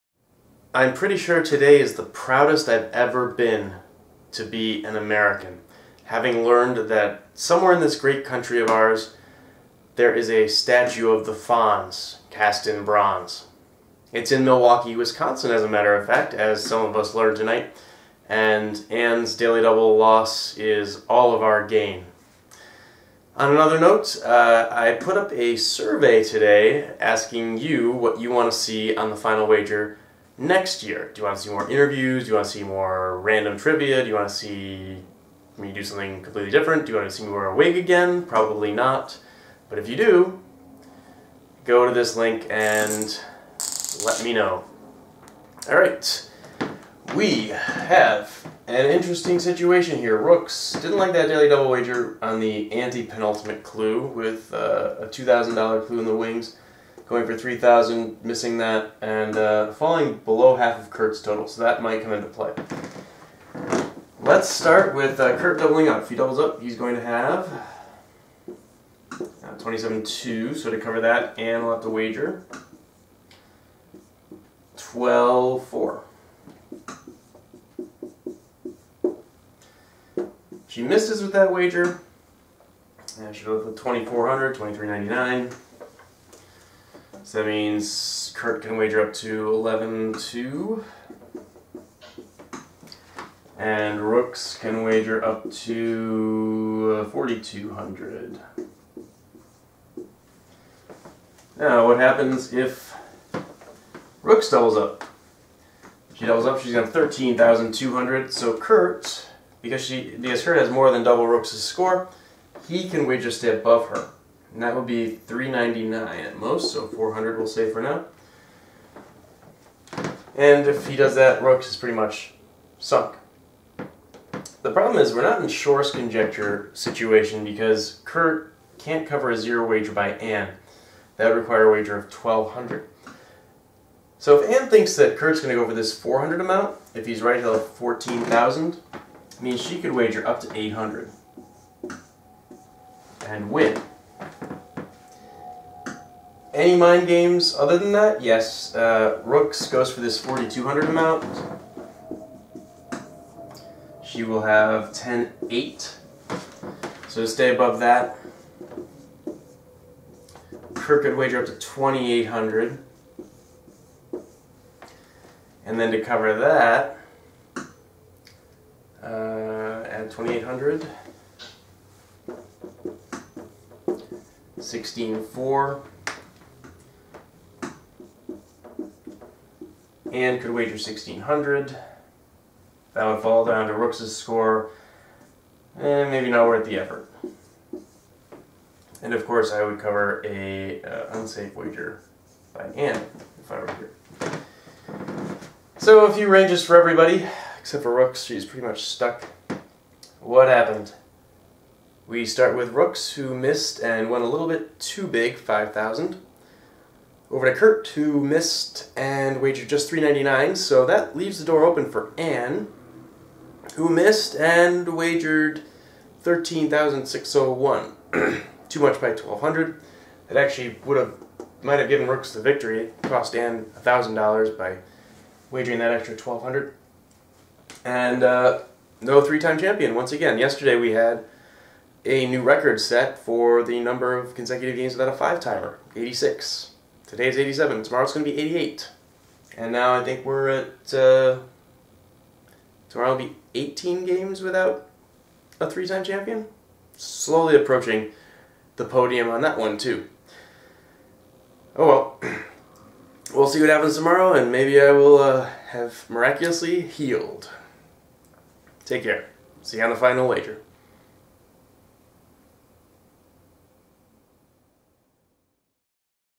The Final Wager – Wednesday, December 17, 2014
2003 Jeopardy! College Champion Keith Williams breaks down the wagering for the match Alysha Rooks vs. Anne Hand vs. Kurt Fritzsche, which first aired December 17, 2014.